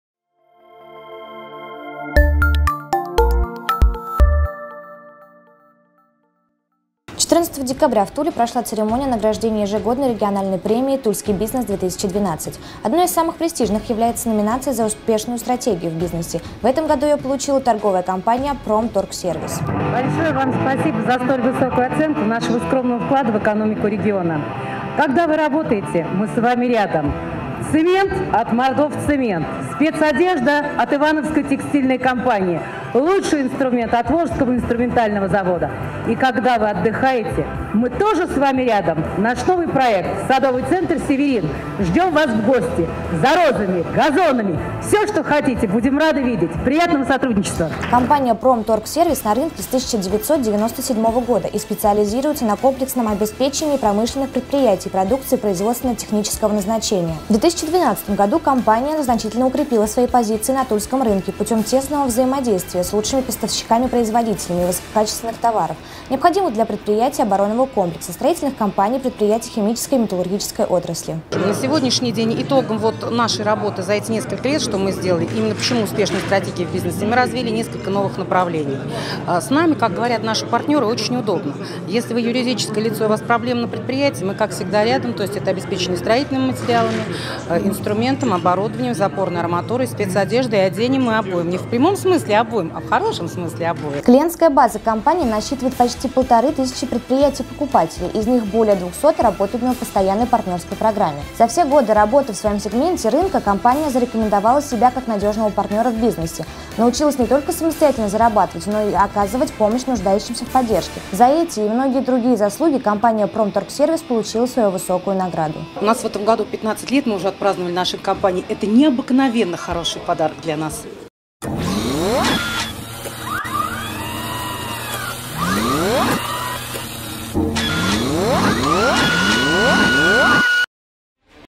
ТК «Промторгсервис»- лауретат «Тульский БИЗНЕС 2012»
14 декабря в Туле прошла церемония награждения ежегодной региональной Премии "Тульский БИЗНЕС 2012". Одной из самых престижных является номинация за успешную стратегию в бизнесе. В этом году ее получила "Торговая компания "Промторгсервис".
Компания "Промторгсервис" на рынке с 1997 года и специализируется на комплексном обеспечение промышленных предприятий продукцией производственно-технического назначения.
В 2012 году компания "Промторгсервис" значительно укрепила свои позиции на Тульском рынке путем тесного взаимодействия с лучшими поставщиками-производителями высококачественных товаров, необходимых для предприятий оборонного комплекса, строительных компаний, предприятий химической и металлургической отрасли.
Клиентская база компании насчитывает почти 1500 предприятий-покупателей из них более 200 работают на по постоянной партнерской программе.  За все годы работы в своем сегменты рынка компания зарекомендовали себя как надежного партнера в бизнесе, научились не только самостоятельно зарабатывать, но и оказывать помощь нуждающимся в поддержке: детскому отделению Тульского областного центра социальной защиты населения, городской больнице No.2, Храму Сергия Радонежского, комбинату школьного питания. За эти и многие другие заслуги компания "Промторгсервис"  получила свою высокую награду.